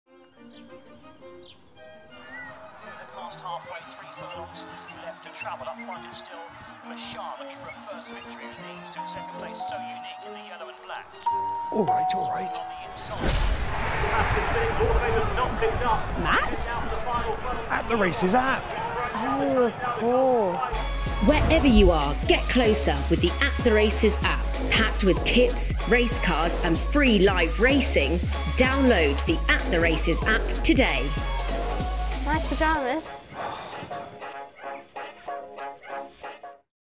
MATT CHAPMAN LIKE YOU'VE NEVER SEEN HIM BEFORE | "NICE PYJAMAS" 😉
Presenters Matt Chapman and Hayley Moore step into the world of acting for the new At The Races app promo. With free live racing, expert tips and the best race cards, download the FREE At The Races app and get closer to the action today… we can't guarantee you won’t end up at a racecourse in your pyjamas though!